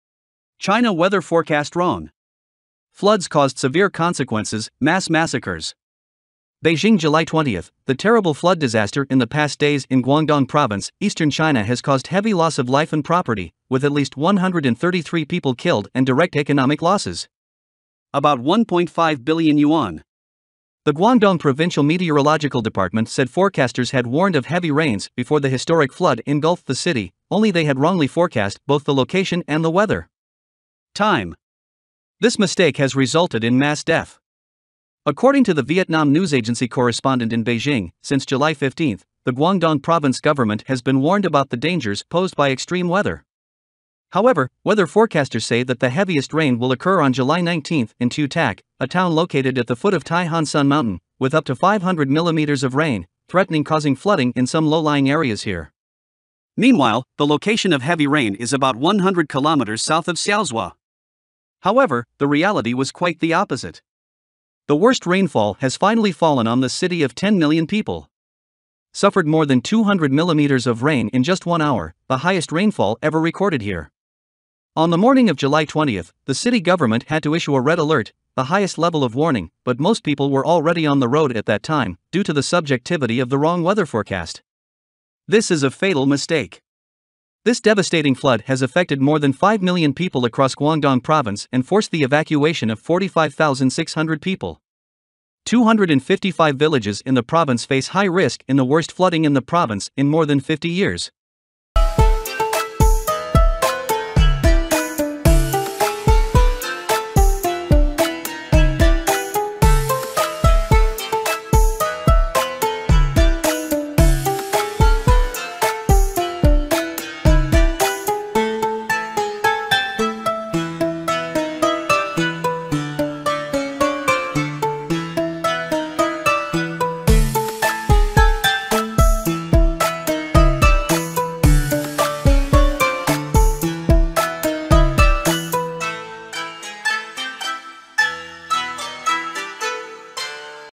China weather forecast wrong! Floods caused severe consequences, mass massacres | Three Gorges dam
China weather forecast wrong! Floods caused severe consequences, mass massacres.
Beijing July 20: The terrible flood disaster in the past days in Guangdong province, Eastern China has caused heavy loss of life and property, with at least 133 people killed and direct economic losses. about 1.5 billion yuan.

The Guangdong Provincial Meteorological Department said forecasters had warned of heavy rains before the historic flood engulfed the city, only they had wrongly forecast both the location and the weather. time. This mistake has resulted in mass death.